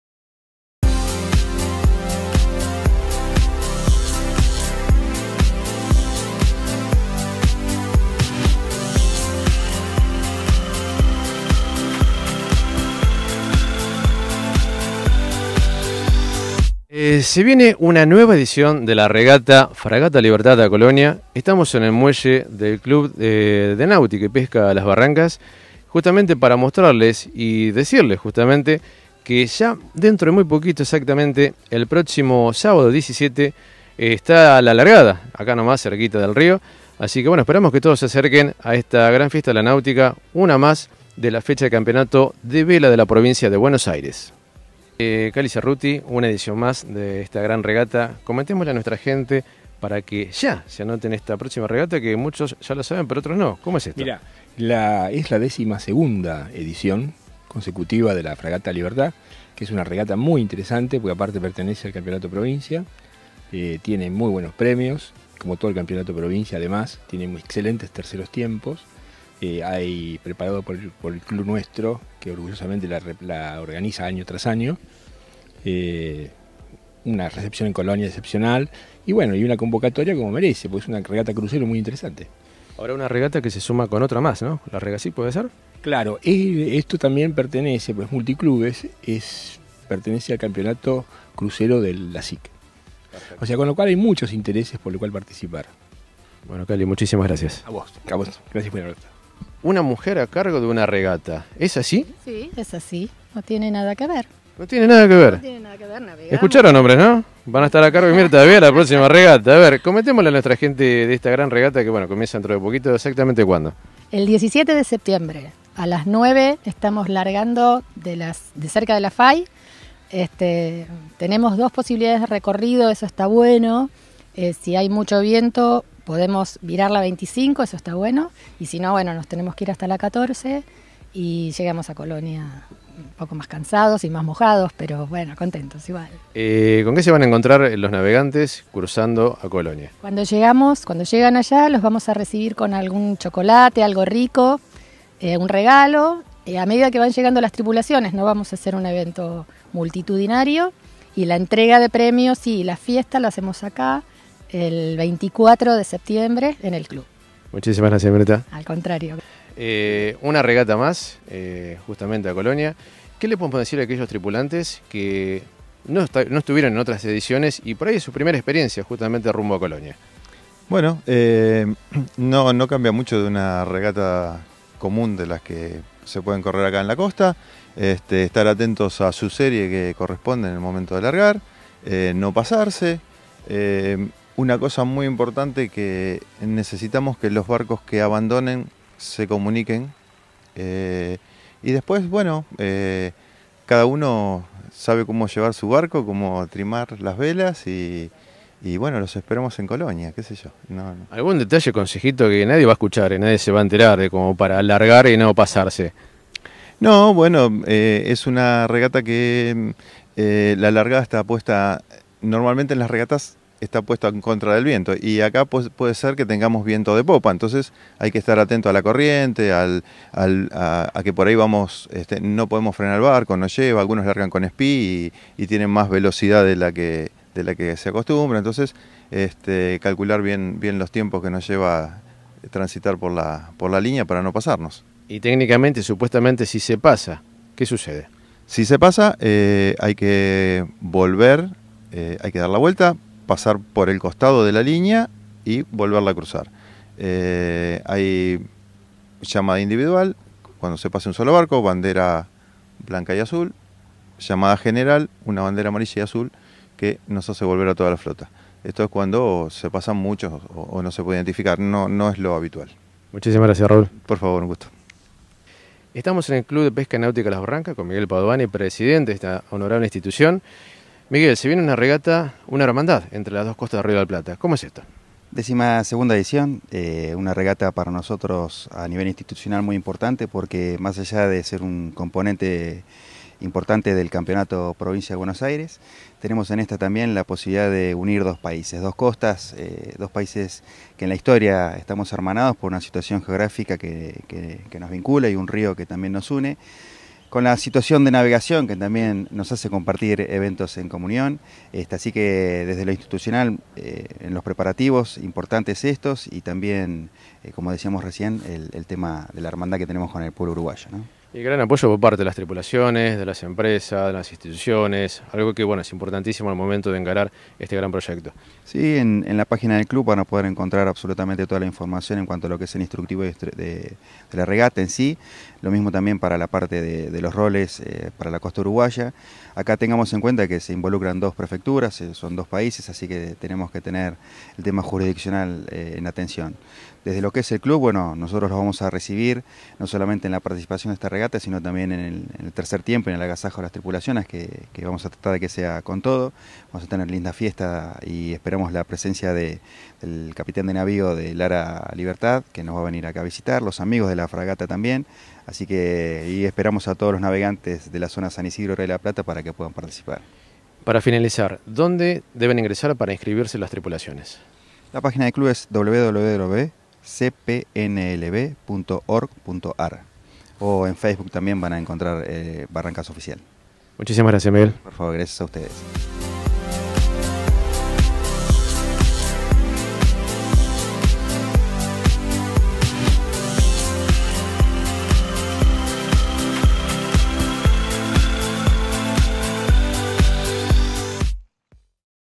17 de septiembre 2016: Se corre la regata Fragata Libertad a Colonia
En esta nota todos los detalles de esta fantástica regata para las series A, B, C, D, E, F y S, Solitarios, Dobles, PHRF/IRC. Conversamos con sus organizadores. Informes